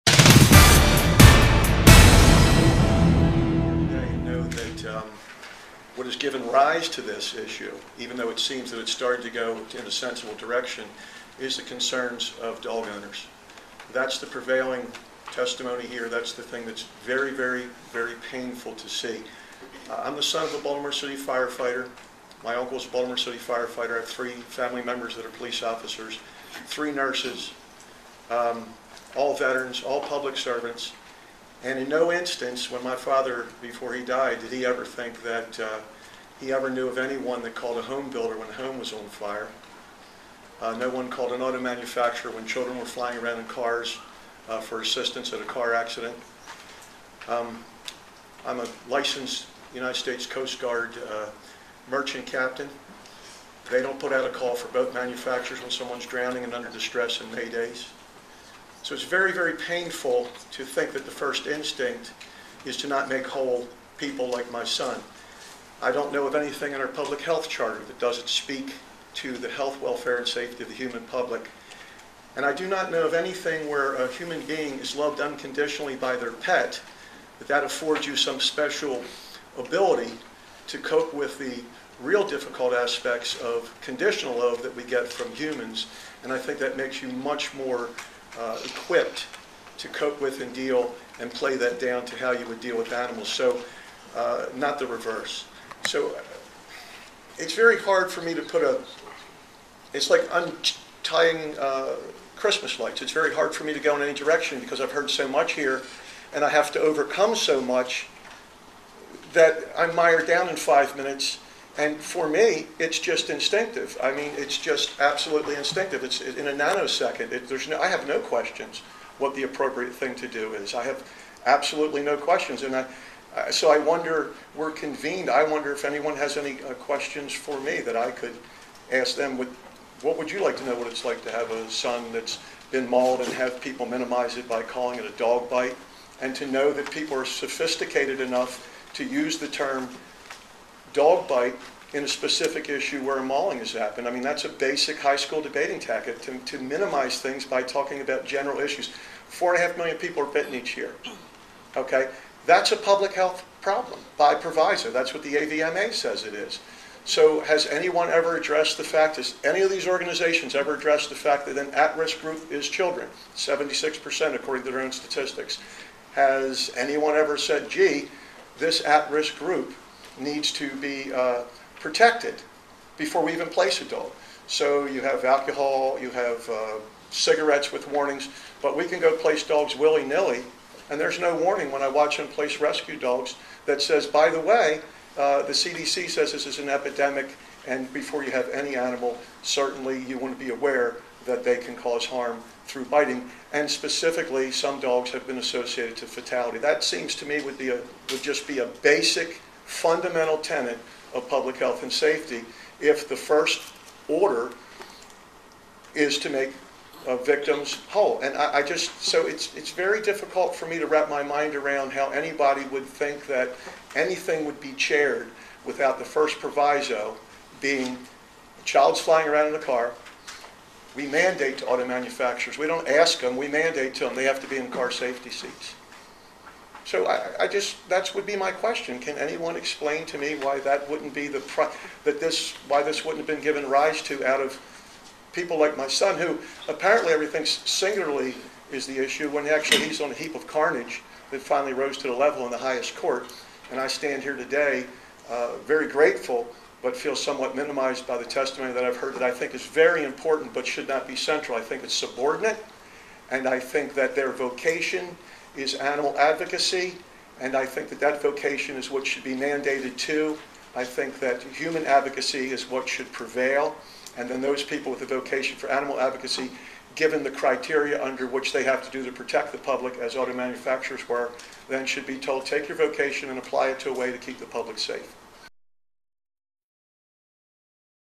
Anthony Solesky Testimony
On Tuesday, June 19, 2012, Anthony Solesky appeared before a special Maryland legislative task force to deal with a Court of Appeals decision defining pit bulls as "inherently dangerous." Solesky was the defendant in the case.  The family sued the owner of the pit bull that attacked their son in 2007, as well as their landlord.